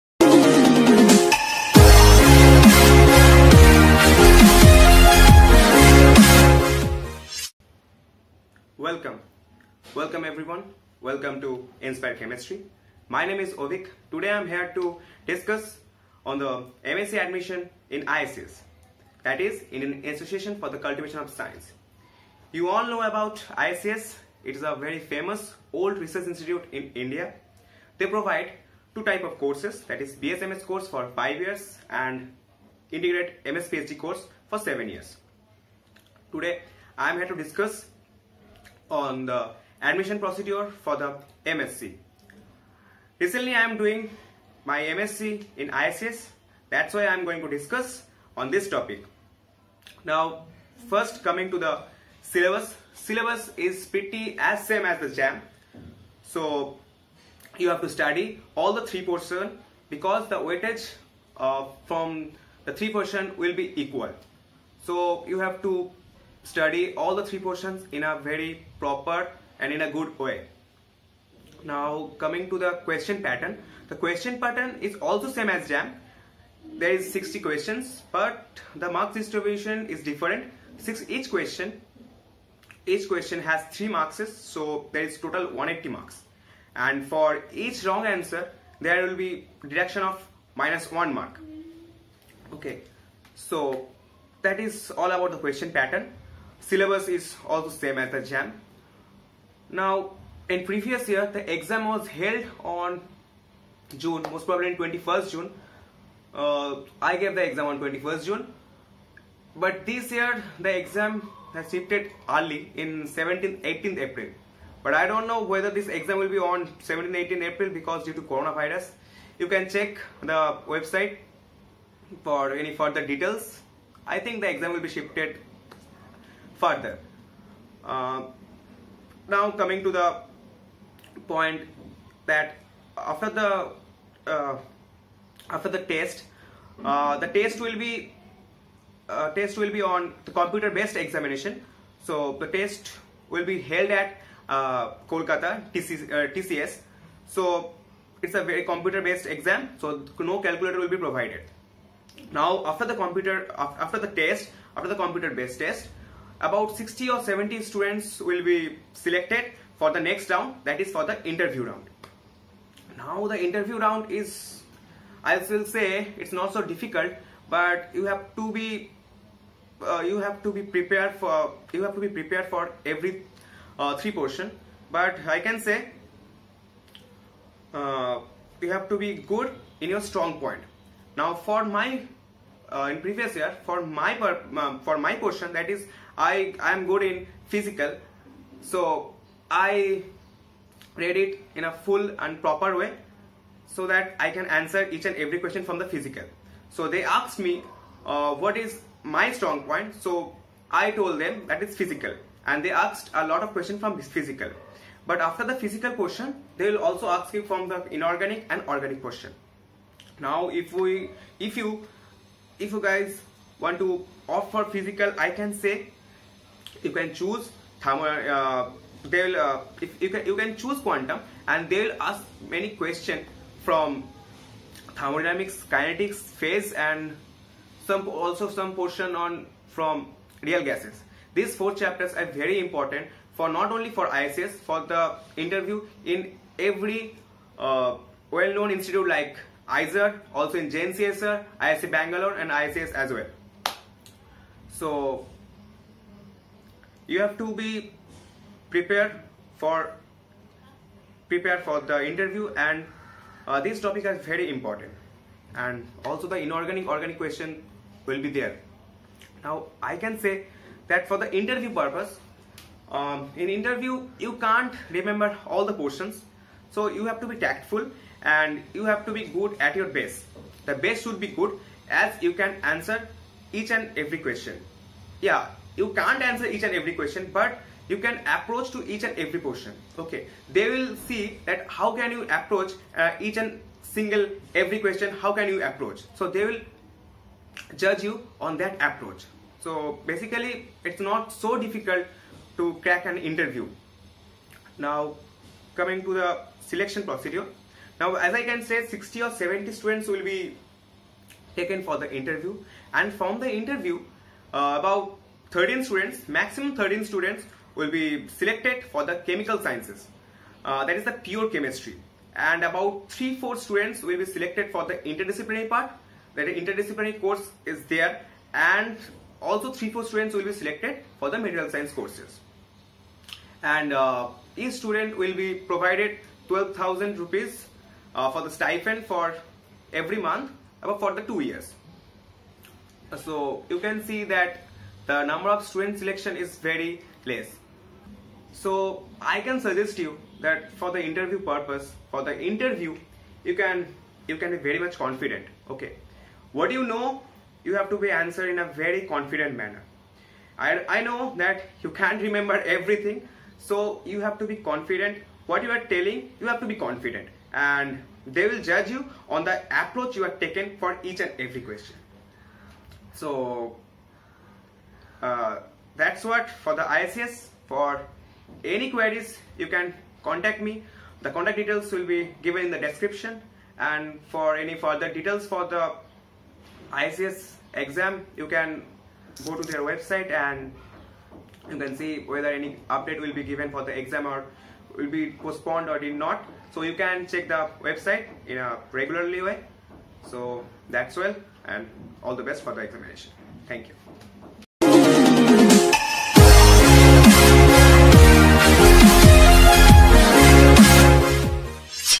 IACS KOLKATA Written exam and Interview Experience || Admission || Important Topics and Questions😊
video is very important for IIT JAM chemistry Aspirants...

In this video we have discussed about IACS KOLKATA written exam and Interview Experience, Admission 2020, Important Topics and Questions Discussion

IIT JAM CHEMISTRY ASPIRANTS
IIT JAM CHEMISTRY 2020
IIT JAM CHEMISTRY 2020CHEMISTRY

Avik Kumar Ojha's (IACS KOLKATA STUDENT)